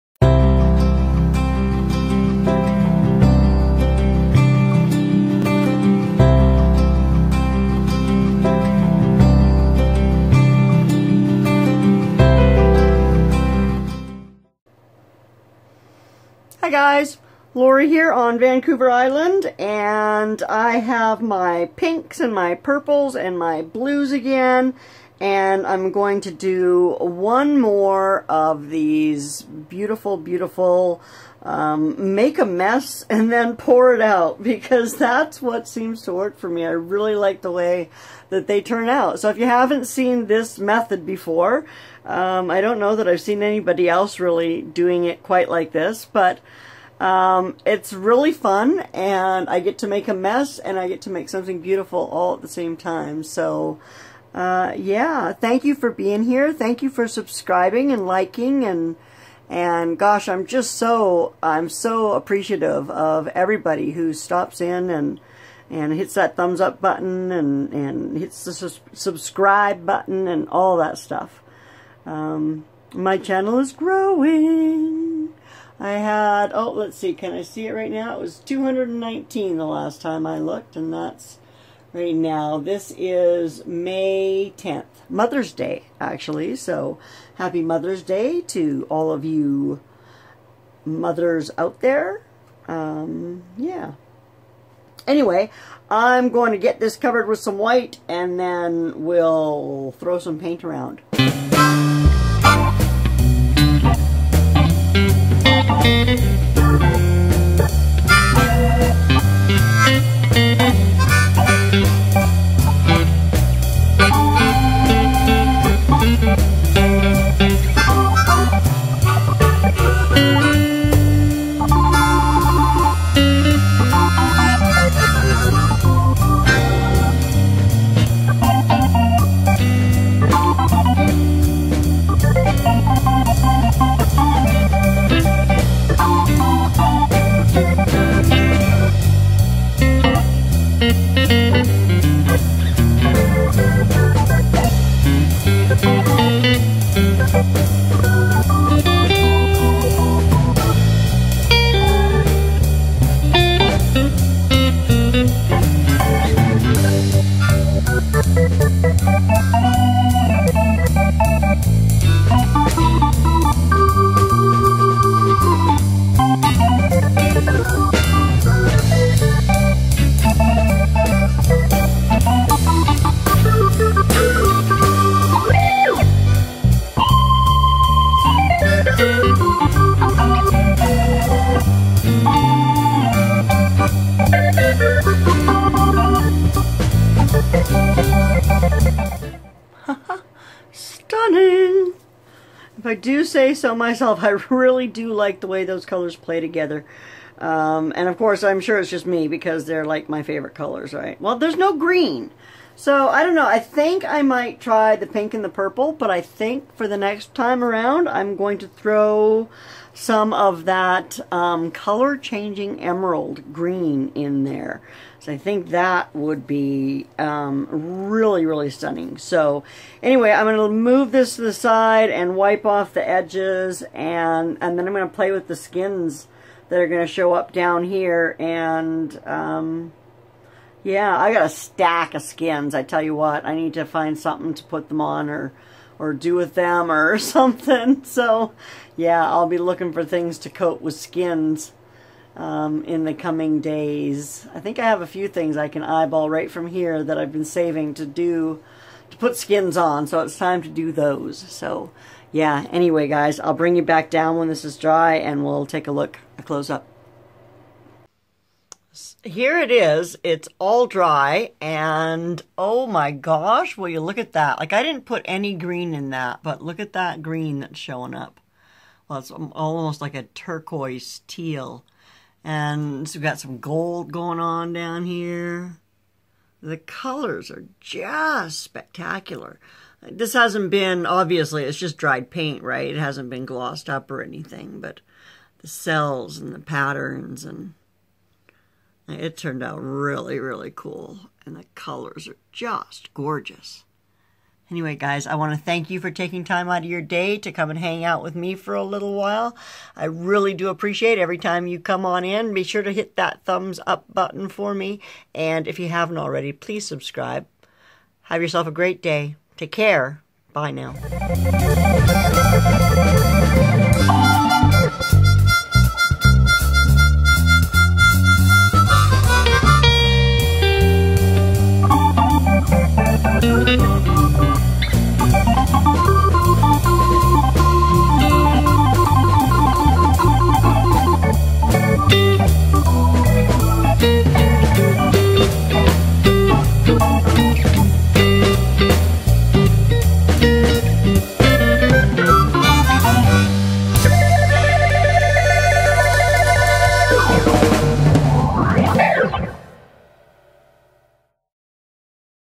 118 Learn to Pour Paint | Pour Paint on a Record | Make a Unique Clock or Wall hanging | Acrylic Art
I just adore making a complete mess, then blowing it out into something beautiful! These colors are stunning together, but definitely adding green next time!

Having fun playing with my new favorite color combo

Colors: Tutti Frutti, Neon Pink, Dioxazine Purple, Thalo Blue, Sky Blue

Cell Activator:
Acrylic paint mixed with equal parts Polycrylic & Floetrol
1/8  tsp wood conditioner (can take quite a while to mix in, but it will)

Paint consistency is a light trace.

Pouring Medium:
Amazon Basics white glue & water 50/50 mix